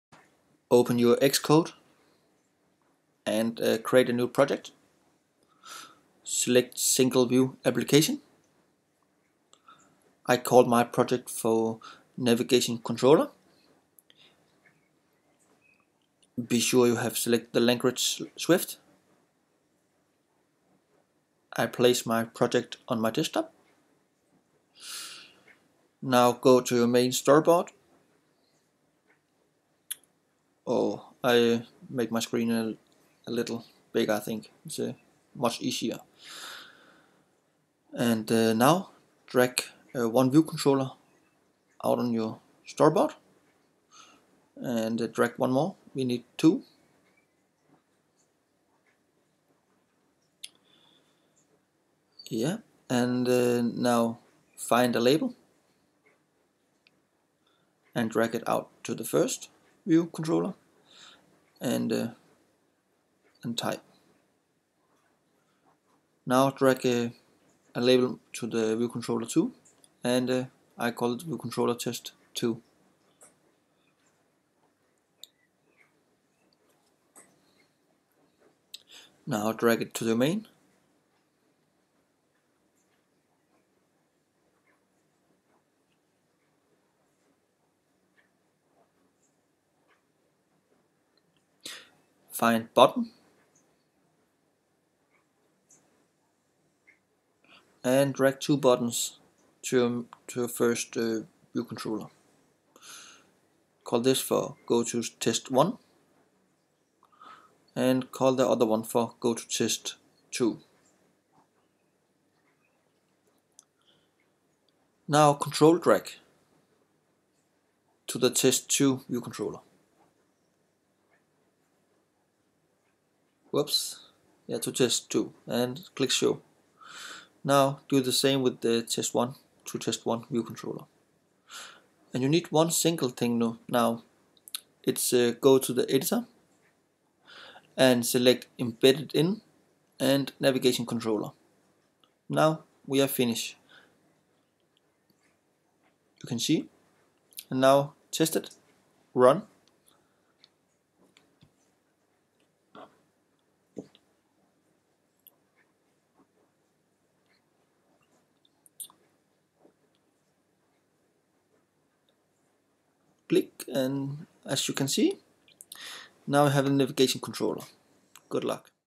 iOS9 - Swift 2.0 - Tutorial - Navigation Controller
This is my first tutorial and sorry for bad English hope you understand what my point is and hope you have great time! :)